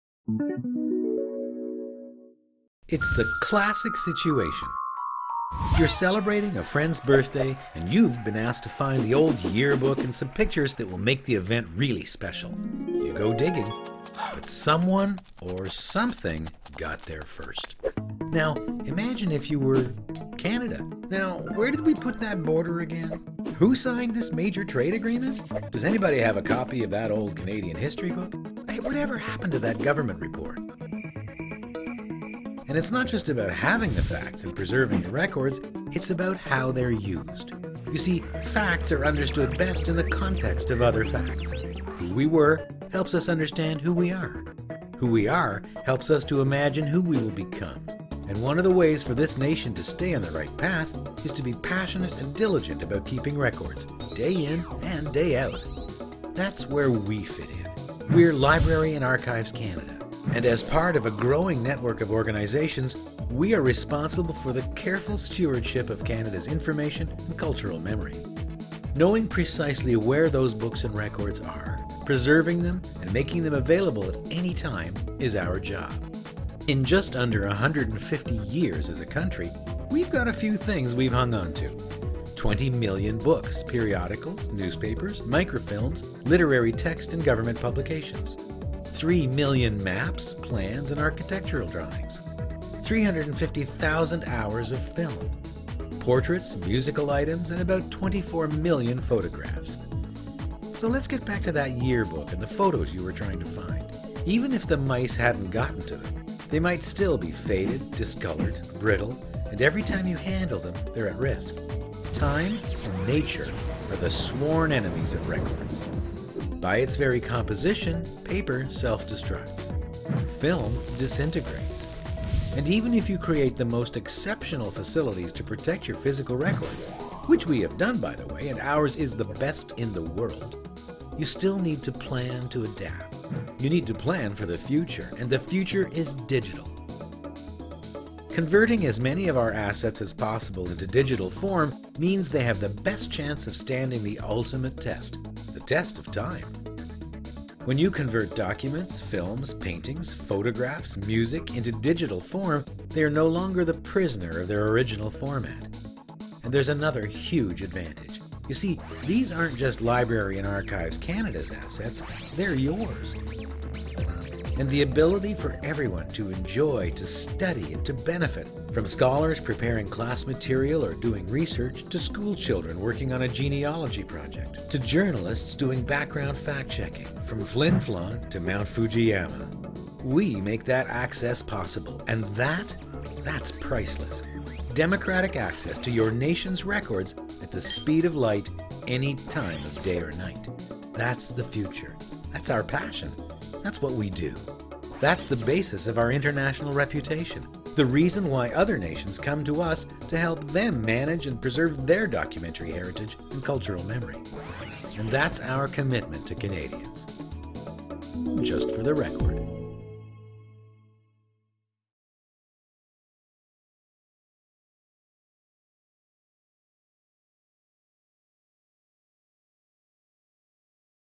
How We Serve Canadians: For the Record
Find out how we care for and safeguard Canada's information resources so that Canadians everywhere can access their documentary heritage now and in the future.